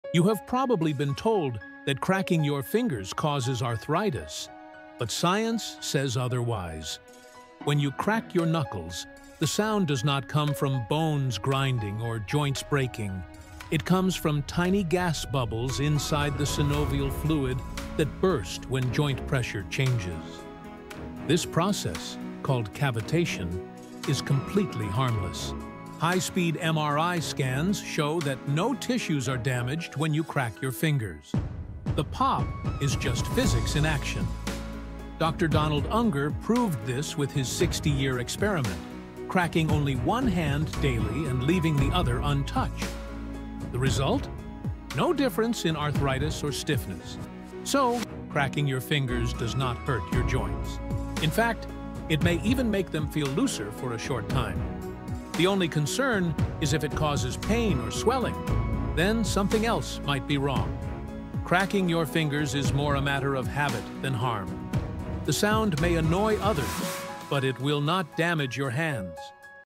Truth About Finger Cracking
Think cracking your fingers causes arthritis? Think again! This short video explains the real science behind knuckle cracking and why it is not harmful. Learn what causes the popping sound, how gas bubbles form in the synovial fluid, and why studies show no connection between cracking and arthritis.